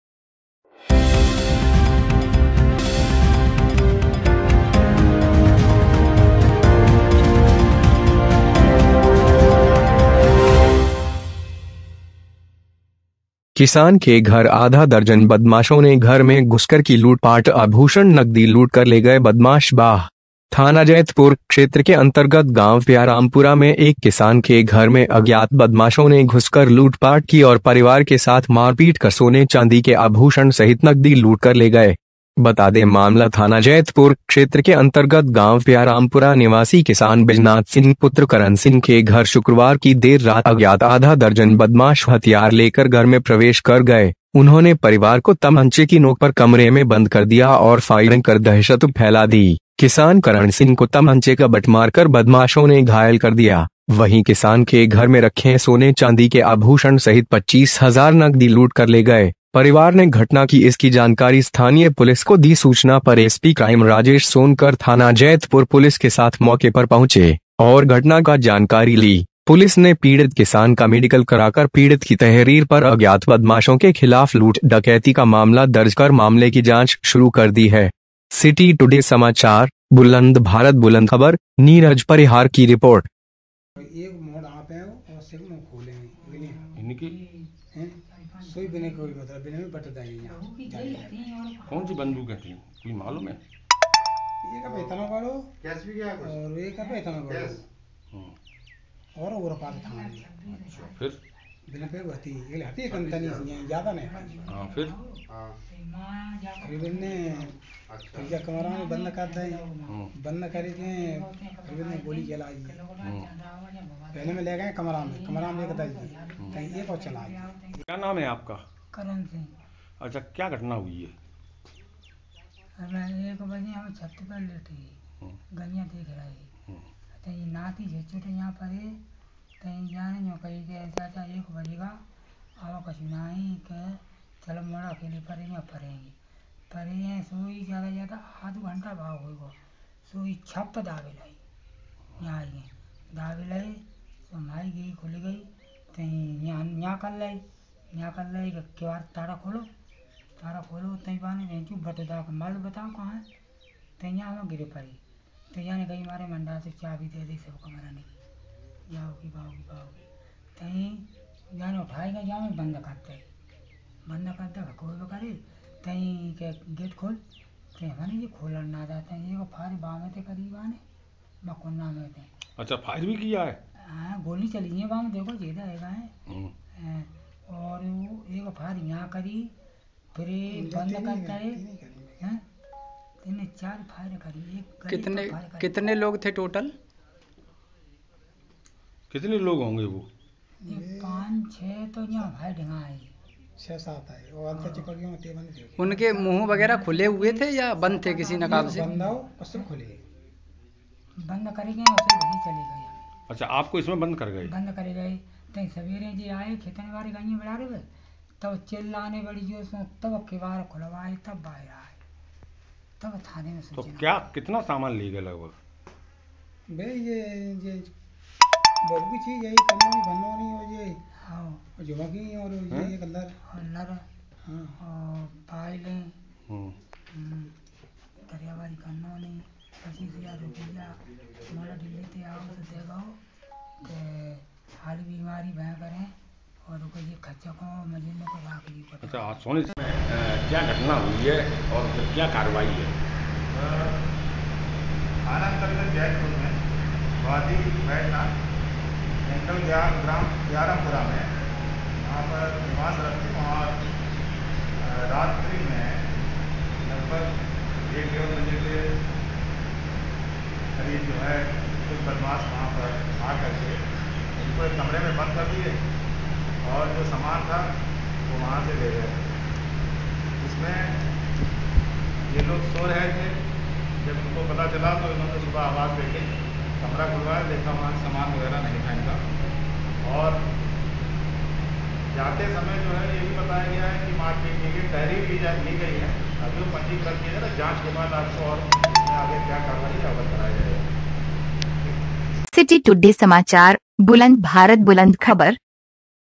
किसान के घर आधा दर्जन बदमाशों ने घर में घुसकर की लूटपाट आभूषण नगदी लूटकर ले गए बदमाश
बाह। थाना जैतपुर क्षेत्र के अंतर्गत गांव प्यारामपुरा में एक किसान के घर में अज्ञात बदमाशों ने घुसकर लूटपाट की और परिवार के साथ मारपीट कर सोने चांदी के आभूषण सहित नगदी लूटकर ले गए। बता दे मामला थाना जैतपुर क्षेत्र के अंतर्गत गांव प्यारामपुरा निवासी किसान बैजनाथ सिंह पुत्र करन सिंह के घर शुक्रवार की देर रात अज्ञात आधा दर्जन बदमाश हथियार लेकर घर में प्रवेश कर गए, उन्होंने परिवार को तमंचे की नोक पर कमरे में बंद कर दिया और फायरिंग कर दहशत फैला दी, किसान करण सिंह को तमंचे का बट मारकर बदमाशों ने घायल कर दिया,वही किसान के घर में रखें सोने चांदी के आभूषण सहित 25 हजार नगदी लूट कर ले गए, परिवार ने घटना की इसकी जानकारी स्थानीय पुलिस को दी सूचना पर एसपी क्राइम राजेश सोनकर थाना जैतपुर पुलिस के साथ मौके पर पहुंचे, और घटना का जानकारी ली, पुलिस ने पीड़ित किसान का मेडिकल कराकर पीड़ित की तहरीर पर अज्ञात बदमाशों के खिलाफ लूट डकैती का मामला दर्ज कर मामले की जांच शुरू कर दी है।